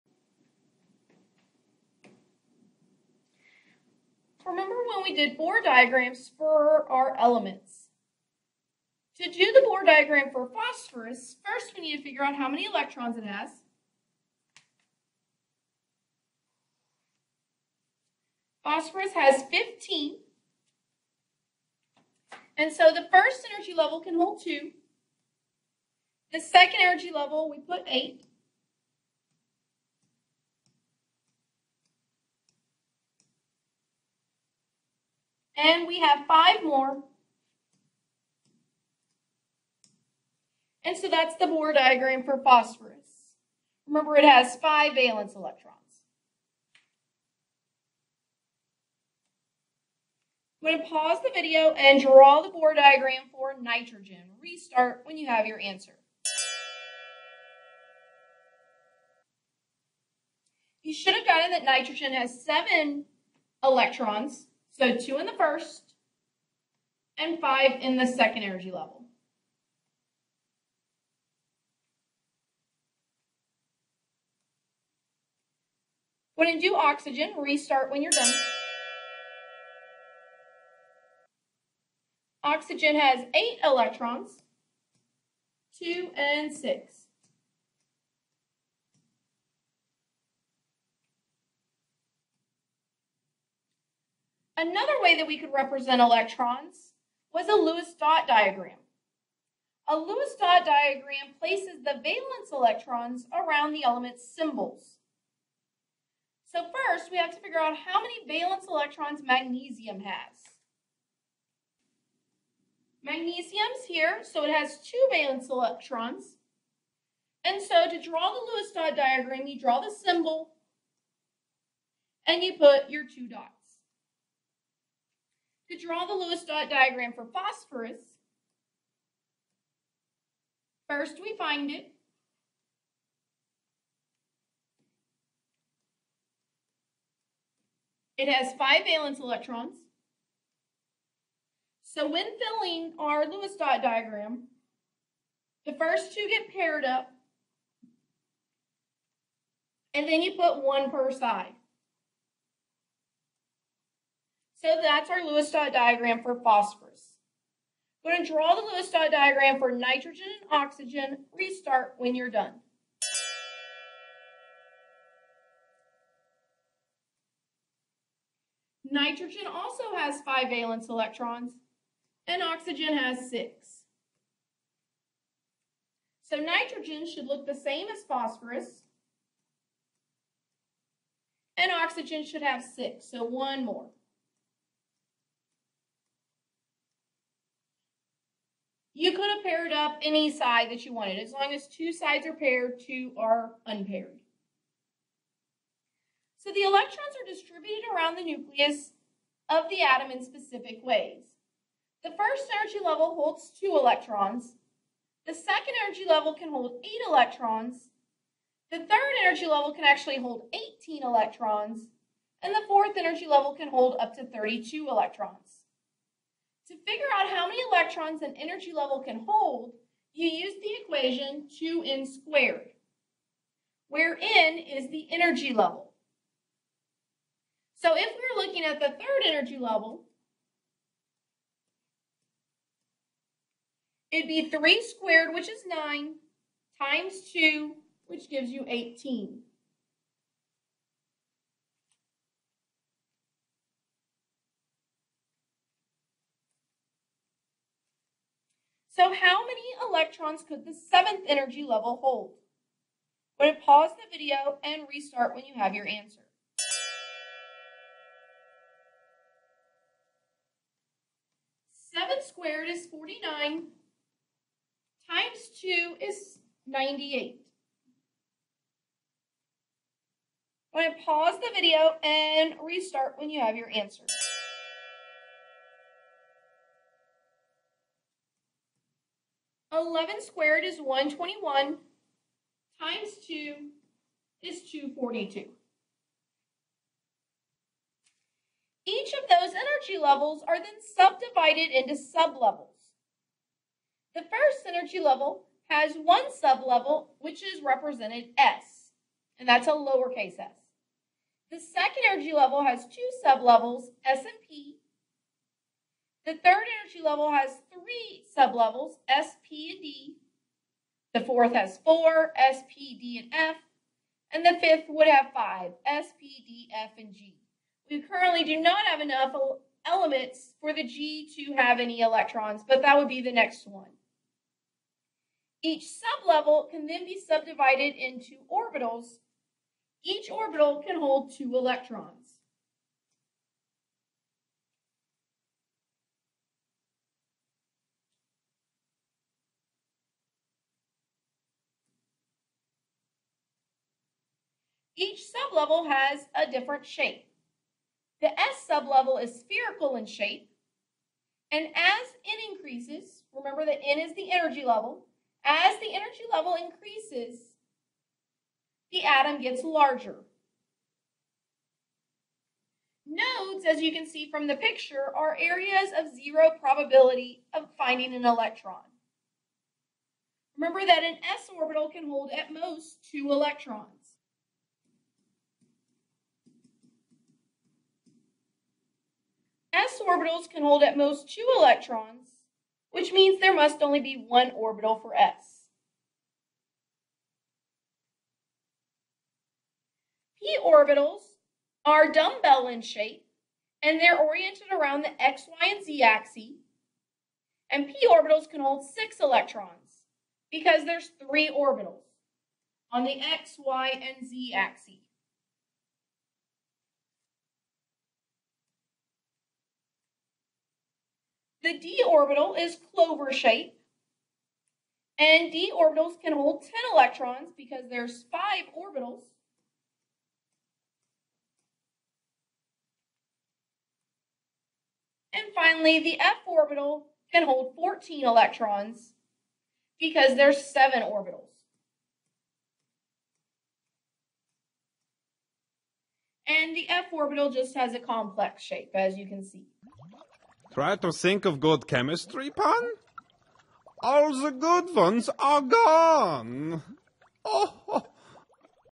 PAP EC part 1:sublevels
This video introduces element lewis dot diagrams and the different sublevels that electrons occupy.